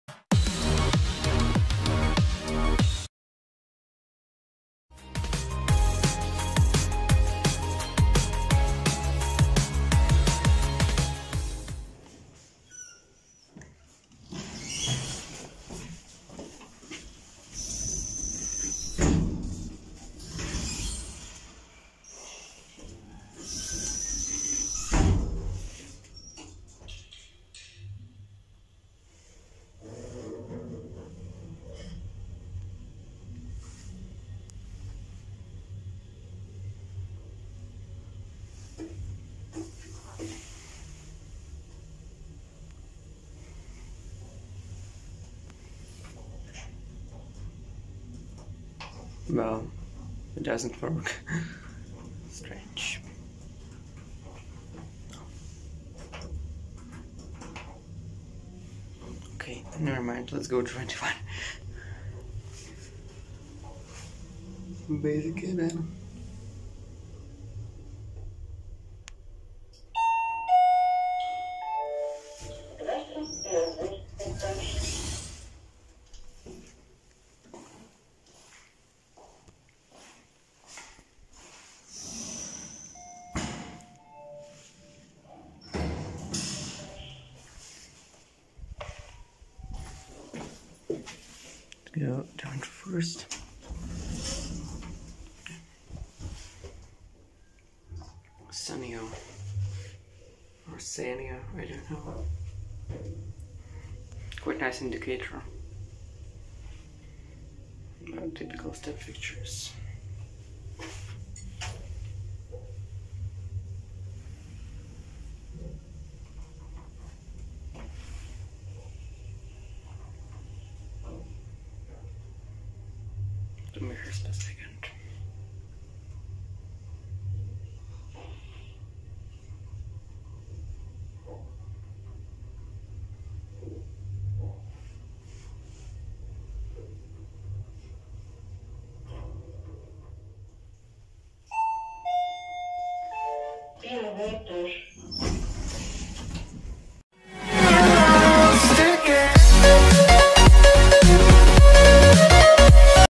Cheap brand-new Sanyo elevators - Izumrudny Apartment Complex (Building 7), Vladivostok, RU
Here we have Sanyo elevators from 2019. They're typical Chinese generic lifts, nothing special at all. They have Shanghai STEP fixtures and interesting white indicators, I think, that they are by same company. Their speed: 105m/min or 1.75m/s is very slow for 22 floors. Overall, these Sanyo elevators are quite bad, better if they installed Hyundai or KONE, or at least Autors, a Chinese elevator brand with Monarch control stations.

RU: Новые лифты Sanyo - Жилой Комплекс "Изумрудный" (Майора Филипова, 7), Владивосток, Россия

Location: Vladivostok, RU

All information about elevator is in the video!
Вся информация о лифте в видео!